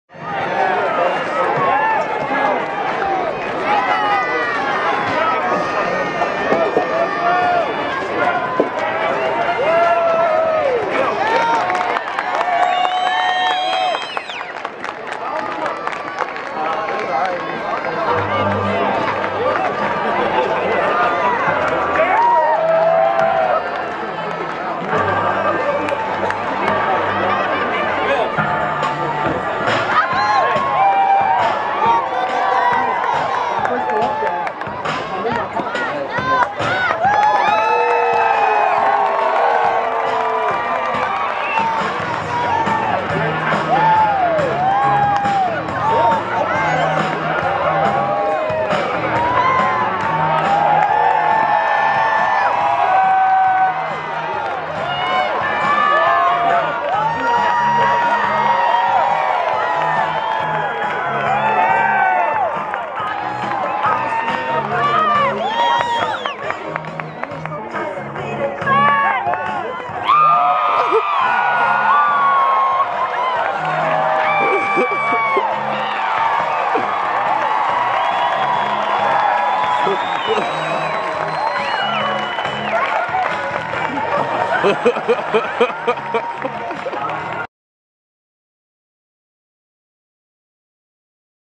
Mascot Dance Off
Sluggo Challenges Eugene Emeralds pitcher Will Scott to a dance off.